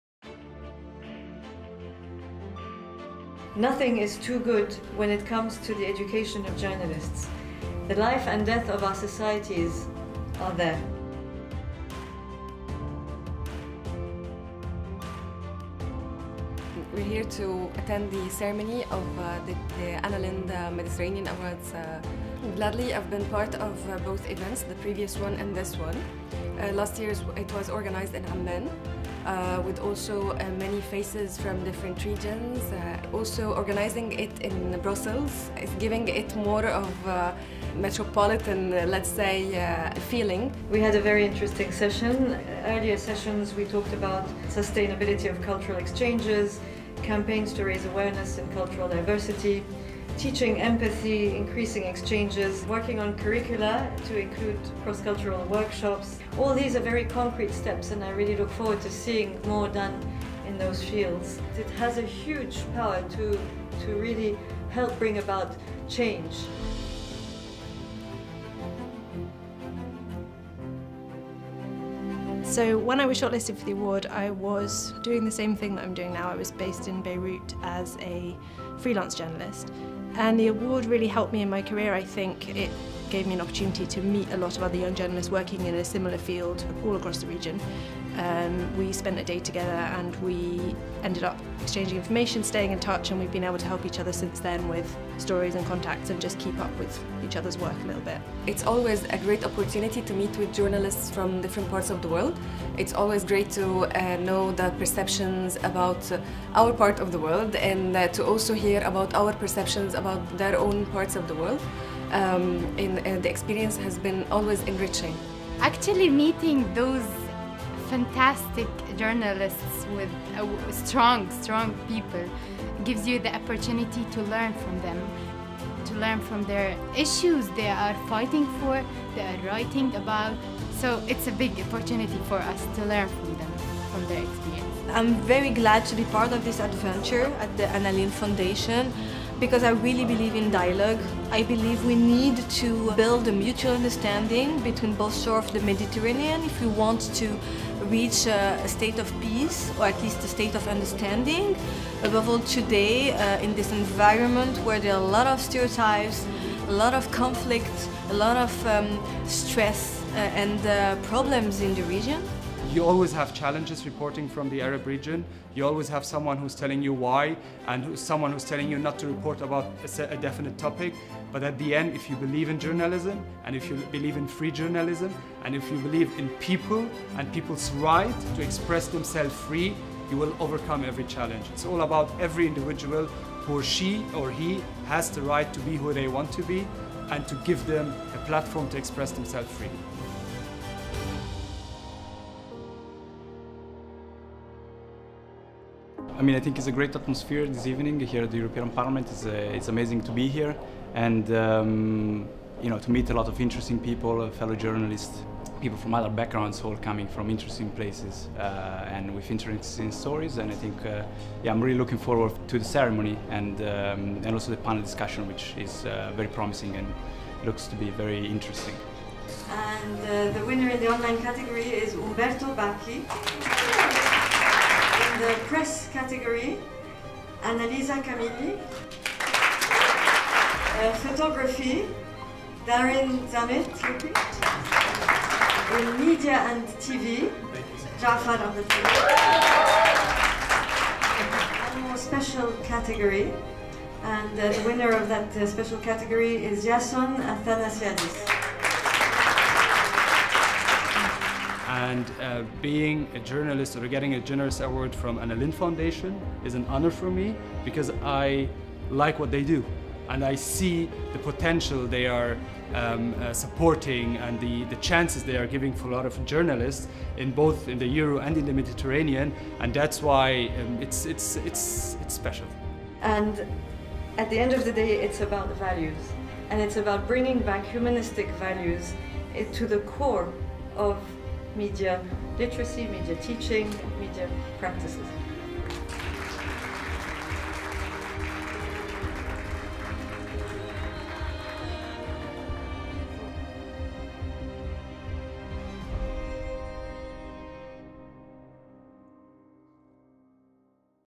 10th Anniversary of the Med Journalist Awards: The Story
10th Anniversary of the Med Journalist Awards took place on 6 December 2017 at the European Parliament in Brussels.
The event consisted of an Alumni Forum that gathered 50+ Journalists, Media Experts with Young Med Voices young leaders and the International Bestowing Ceremony where HRH Princess Rym Ali presented the Awards to the winners.